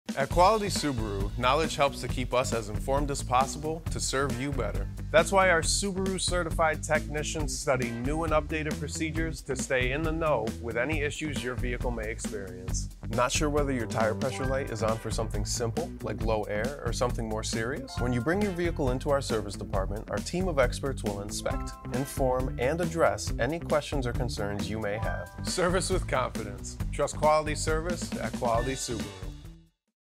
We’re Here to Serve You Better!
Get service with confidence when you bring your vehicle to the Quality Subaru service department, where our team of expert technicians will help keep you on the road for many miles to come!